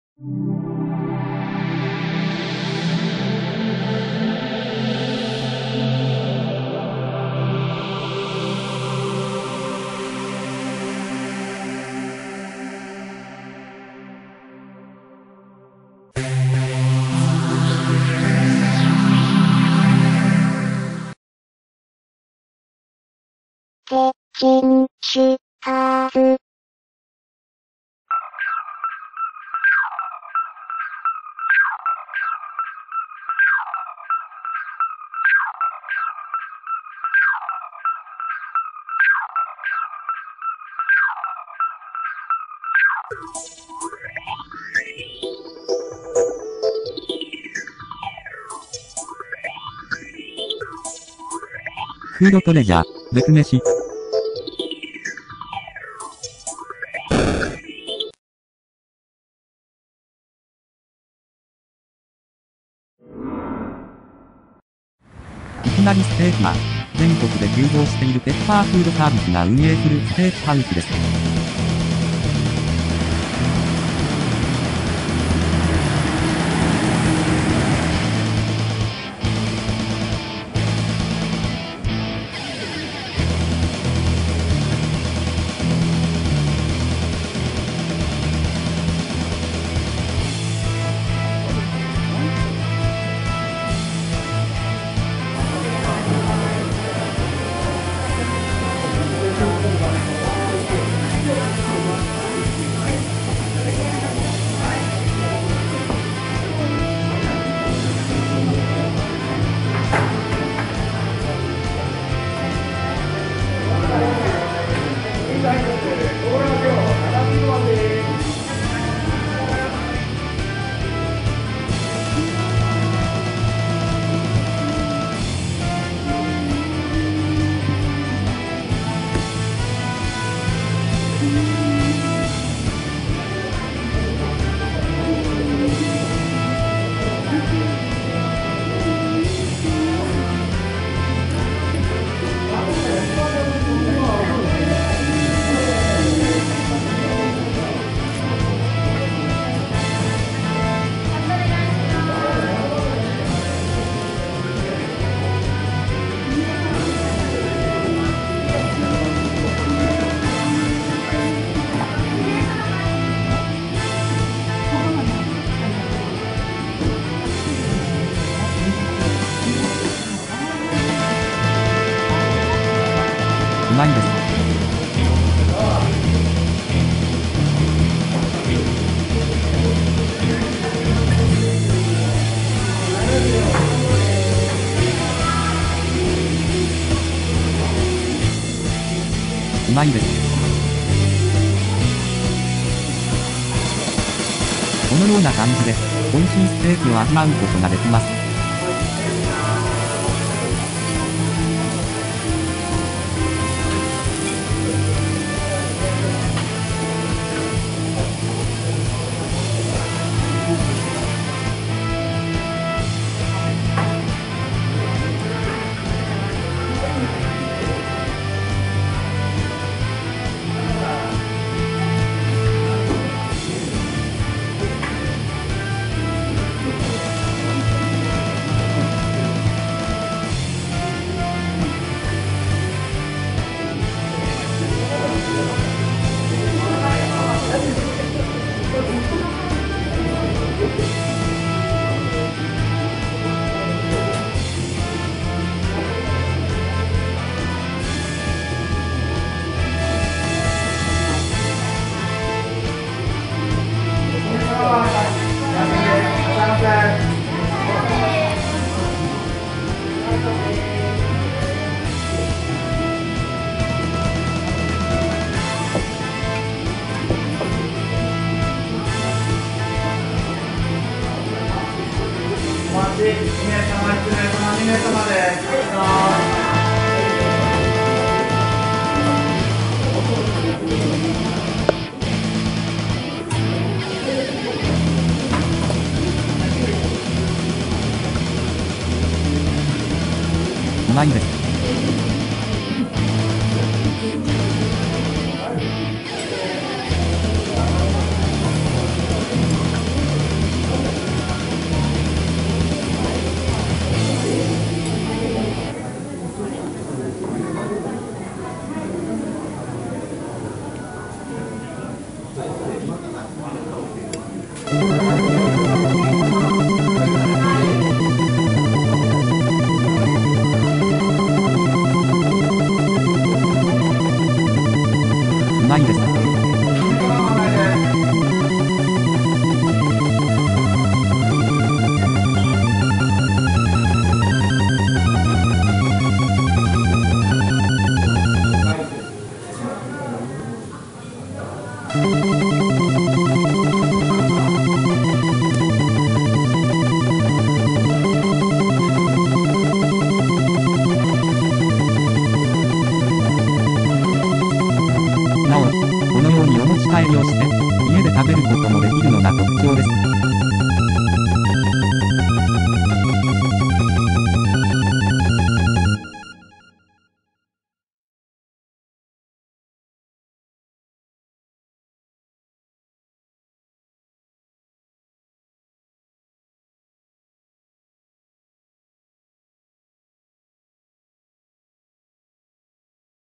Food Tresures- ベクめしっ！#27
※ 本作品は、日本語と英語の字幕が双方に表示されます。 ※ Japanese and English subtitles are shown by both to this work.
日常で溢れている様々な飲食店を紹介する本家M.o初の料理番組。 第２７回目の今回は、全国各地で急速に店舗数を拡大したステーキハウス「いきなり!!ステーキ」を紹介します。
様々なメニューをまるで自分で食べているかのように体感できる新感覚の料理番組。
2018年12月24日(月)公開。